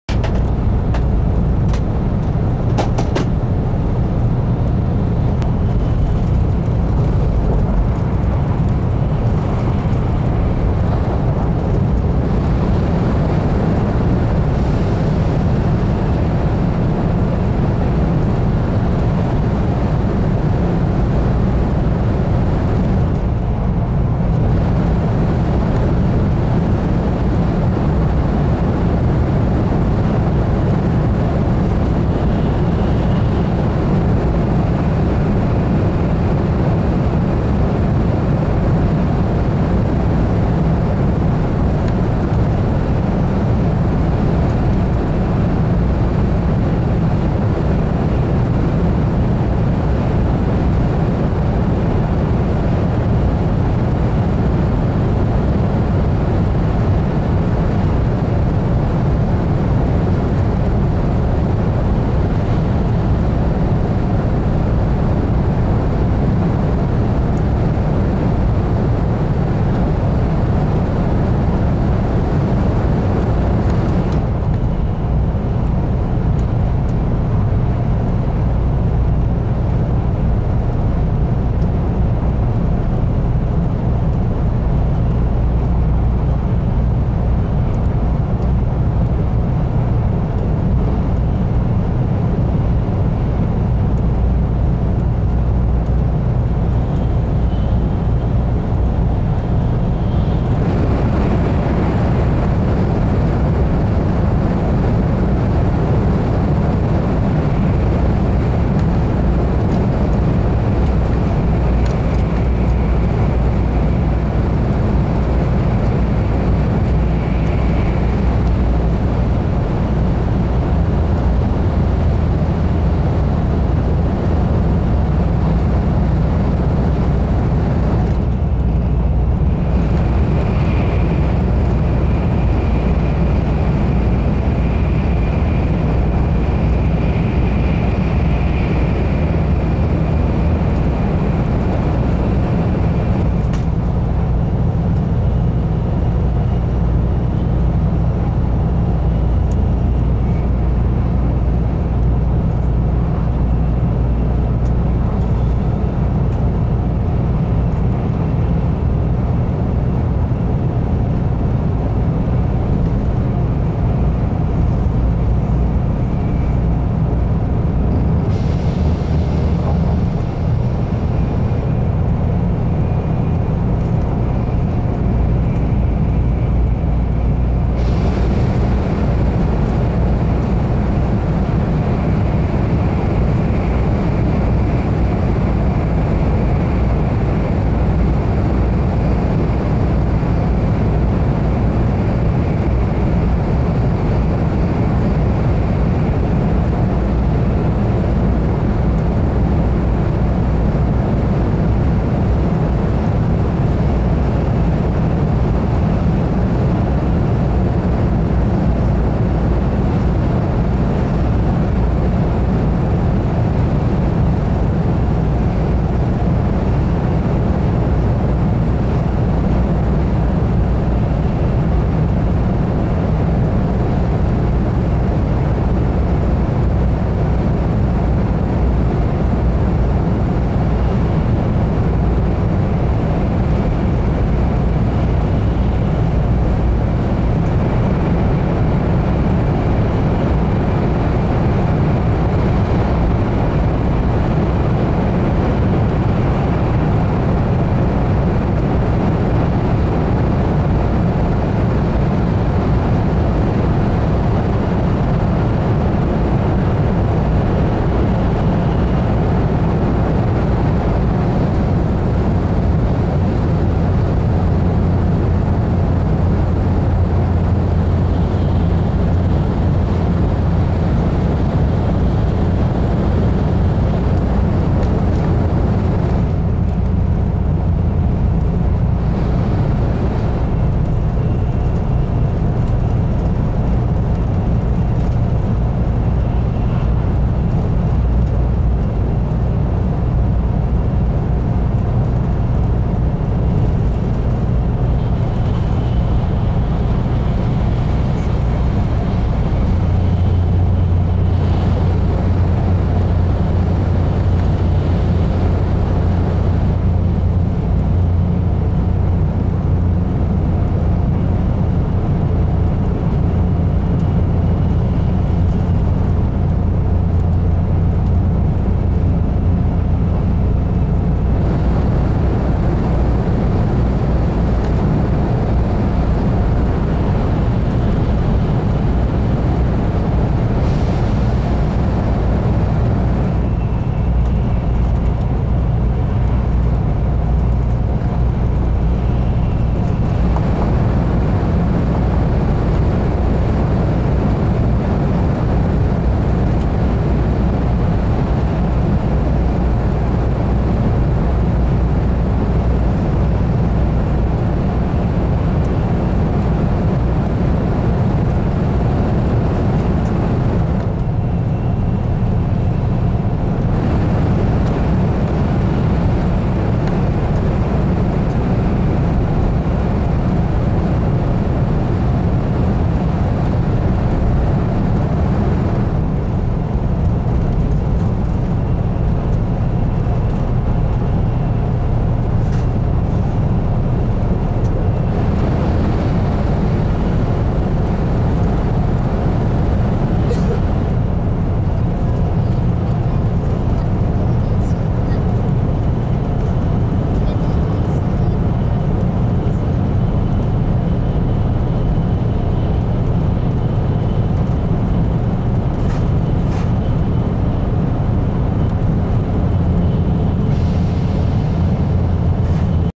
【完全走破27】500系のぞみ29号北側車窓展望 相生-岡山その1
500系で運行されているのぞみ29号東京－博多間、北側（山側）の車窓展望です。相生からは山陽新幹線らしく山々をまっすぐトンネルで突き抜けていく経路をとります。ところどころ瀬戸内地方でよく見られる溜池があります。
Shinkansen  from Tokyo to Hakata on service.  Even the series 500 has stepped down from the service between Tokyo and Shin-Osaka, still most popular Japanese train in the world.